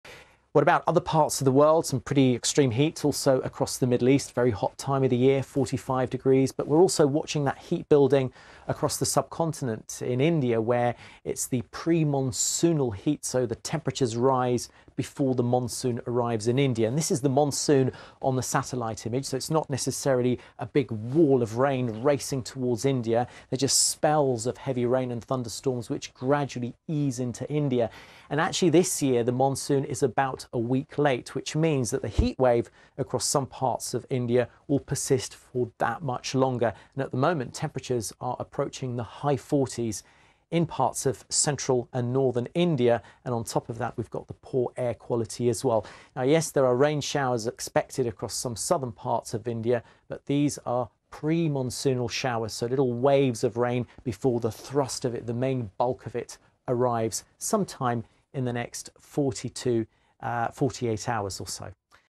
Weather Events 2019 - The heat (Arabia & India) - BBC News - June 2019
The heat in the Arabian (and Middle East) area that will affect the countries in that region and India, waiting for the first of the Monsoon rains, as the heat wave continues.